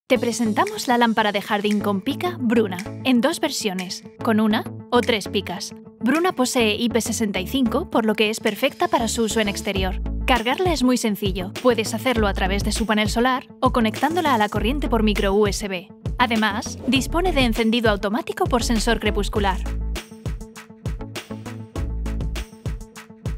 Lámpara de jardín con pica BRUNA | Newgarden
Te presentamos la lámpara de jardín con pica BRUNA en dos versiones: con una o tres picas. Bruna posee IP65 por lo que es perfecta para su uso en exterior. Cargarla es muy sencillo, puedes hacer a través de su panel solar o conectándolo a la corriente por Micro USB.